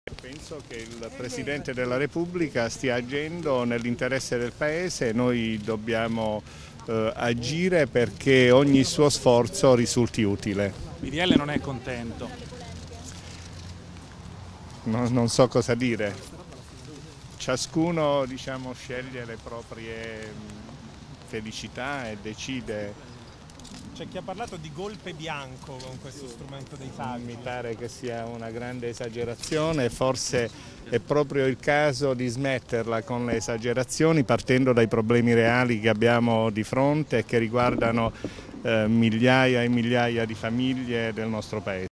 Governo, Bubbico: condividere priorità non è fare governissimo
Dichiarazione di Filippo Bubbico, senatore Pd